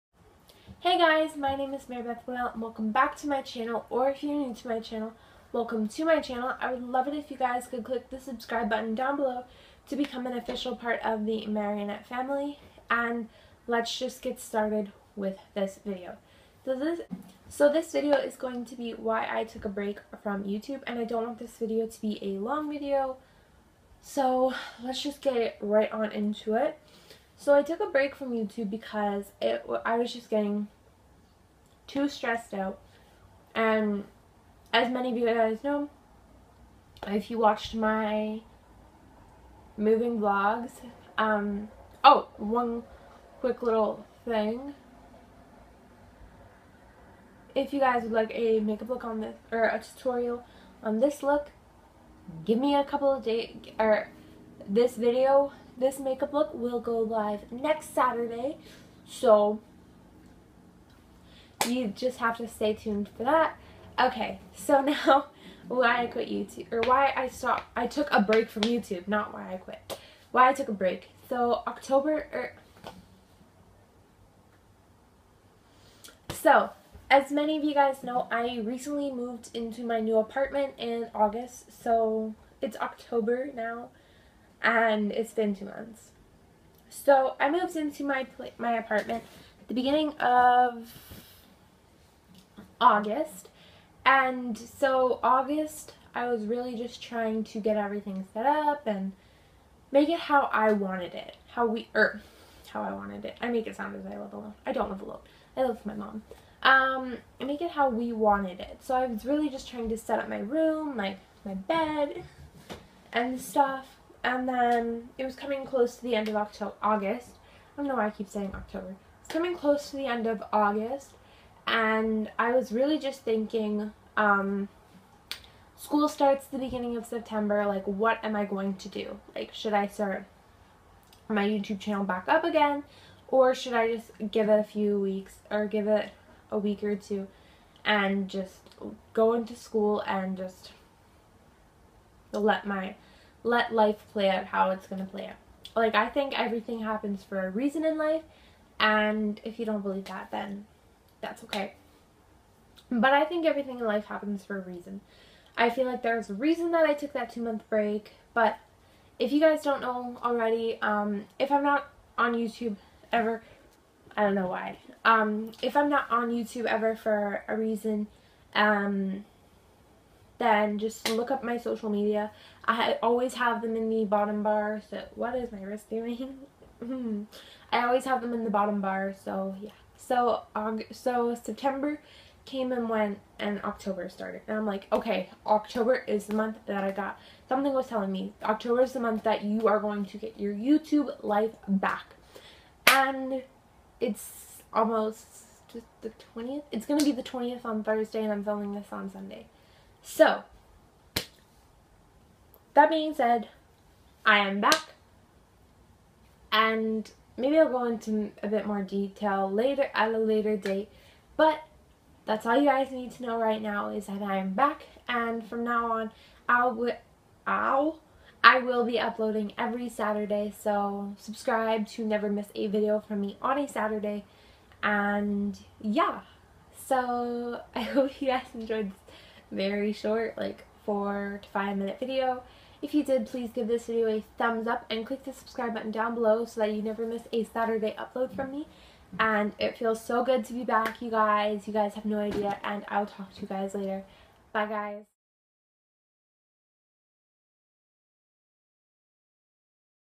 Why I took a break
Hey guys

So today's video is going to be why I took a break from YouTube. Now I know I didn't go into too much detail but maybe at a later date i will.

Xoxo
Marybeth waddell